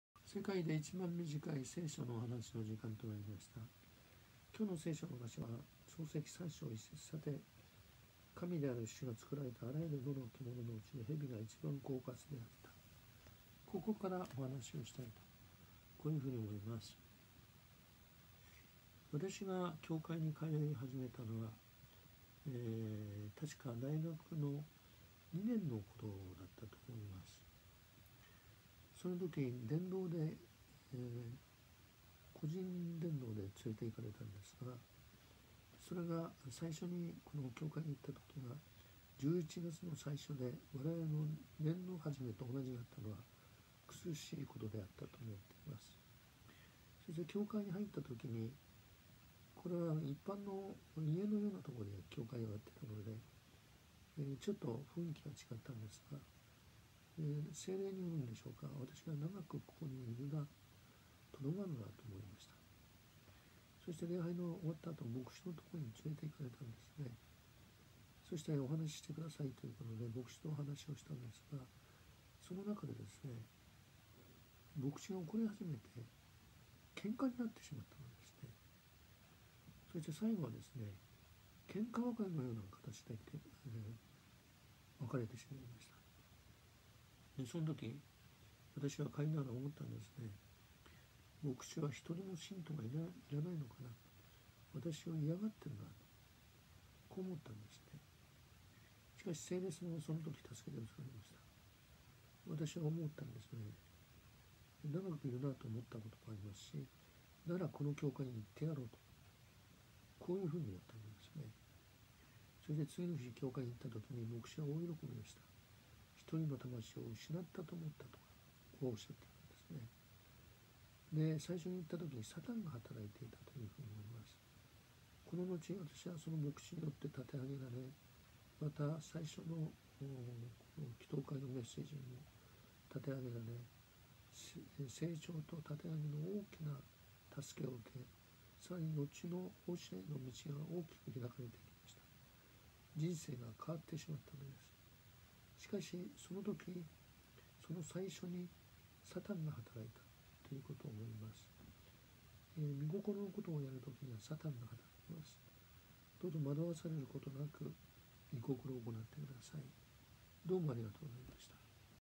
世界で一番短い聖書のお話 NO.631
さて、神である主が造られたあらゆる野の獣のうちで、蛇が一番狡猾であった。
創世記　3章1節